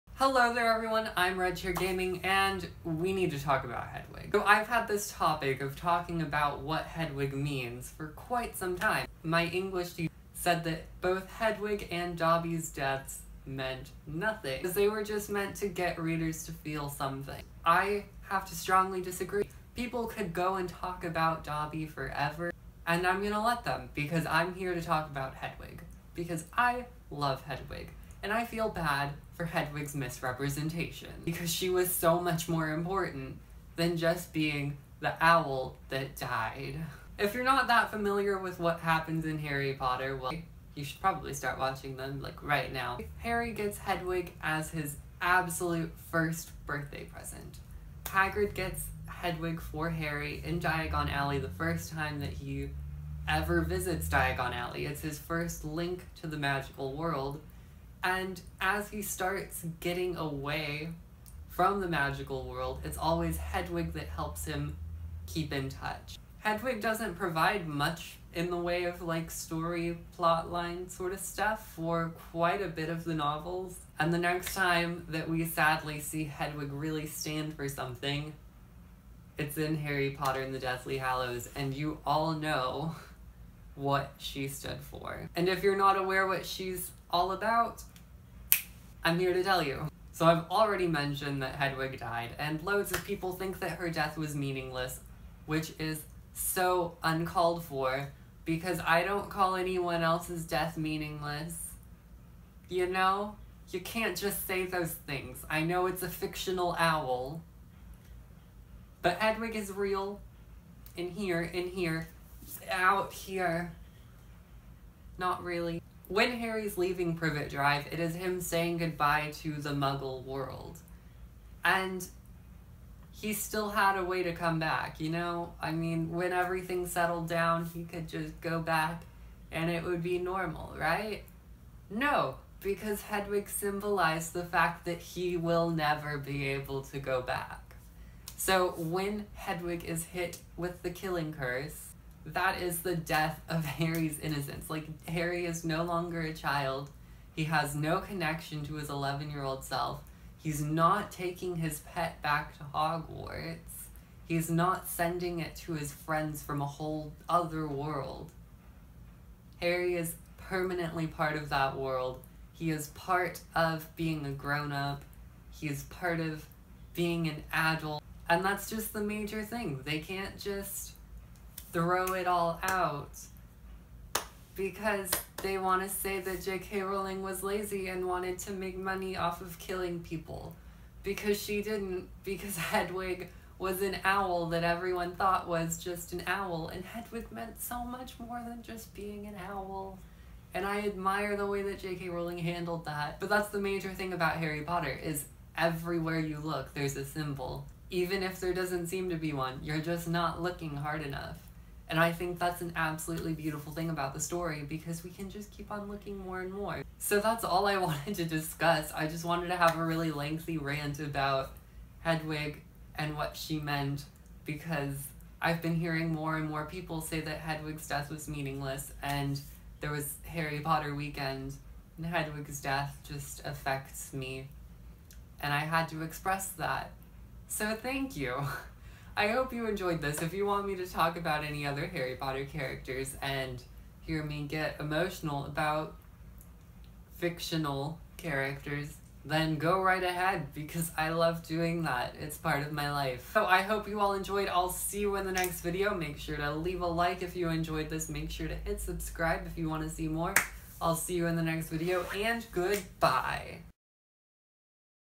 Emotionally Ranting About Hedwig - Yes, the Owl
Sometimes I need an emotional rant.